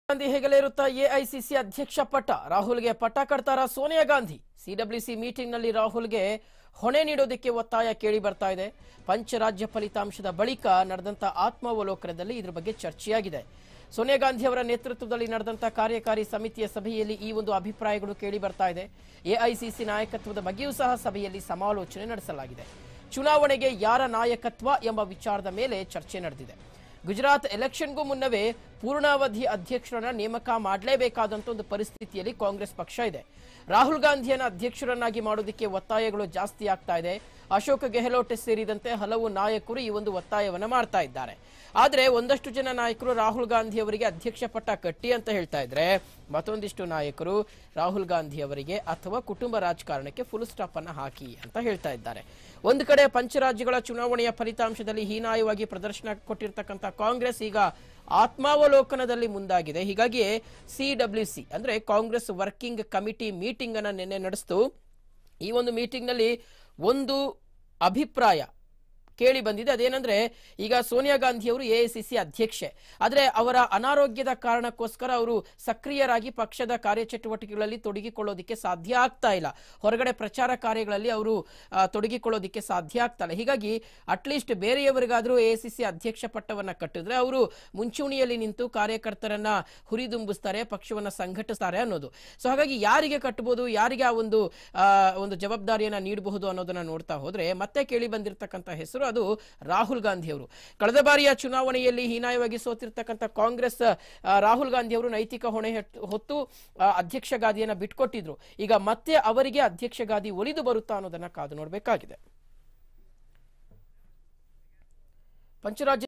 ಮತ್ತೆ ರಾಹುಲ್​​ ಹೆಗಲೇರುತ್ತಾ AICC ಅಧ್ಯಕ್ಷ ಪಟ್ಟ.. ರಾಹುಲ್​​ಗೆ ಪಟ್ಟ ಕಟ್ತಾರಾ ಸೋನಿಯಾ ಗಾಂಧಿ.! AICC
Btv News Kannada   ಮತ್ತೆ ರಾಹುಲ್​​ ಹೆಗಲೇರುತ್ತಾ AICC ಅಧ್ಯಕ್ಷ ಪಟ್ಟ.. ರಾಹುಲ್​​ಗೆ ಪಟ್ಟ ಕಟ್ತಾರಾ ಸೋನಿಯಾ ಗಾಂಧಿ..! AICC |Rahul Gandhi | Sonia Gandhi |